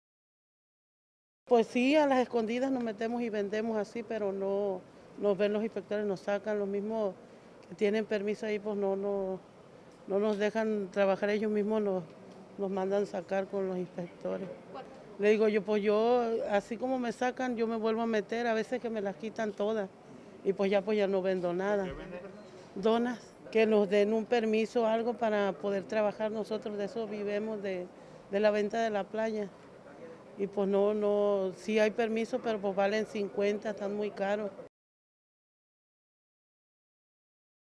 Vendedores de playa conforman cooperativa 2
83 Vendedores de playa están conformando una cooperativa con la intención de poder regularizar su actividad comercial y a su vez tener acceso a distintos apoyos por parte del gobierno, esta mañana un grupo representante acudió al ayuntamiento para solicitar el apoyo de la autoridad municipal para agilizar el proceso.
Además de regularizarse pretenden hacer la diferencia siendo una cooperativa sustentable que cuida las playas logrando hacer conciencia entre los trabajadores por medio de capacitación, así lo informo el Jorge Ramos líder de esta cooperativa.
Otra petición a las autoridades competentes es el apoyo para poder obtener sus permisos ya que aseguran que regularmente son víctimas de actos de hostigamiento por parte de inspectores de oficialía mayor quienes les quitan sus productos que son esfuerzo fabrican para lograr obtener un recurso económico para llevarle a sus familias, tal es el caso de la señora Zenobia Sarmiento.